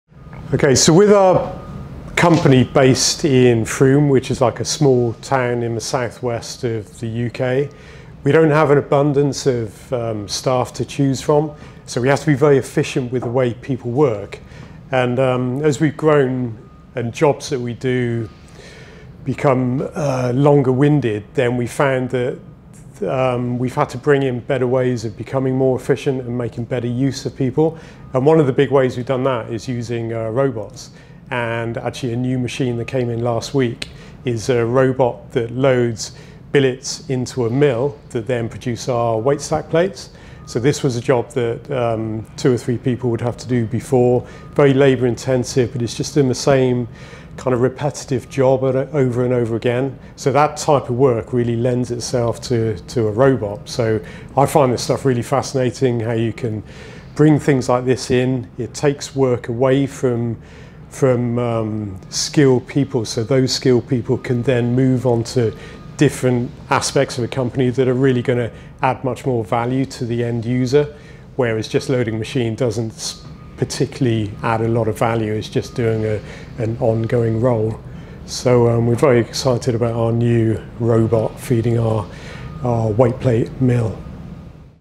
Robots to Free Up Skilled Workers - Watson Gym Equipment
We have just had a new Robot at Watson Gym Equipment. This machine loads and unloads our CNC Mill which will create our Weight Stacks. Simon explains the true reason to why we are looking to robots as we grow and expand.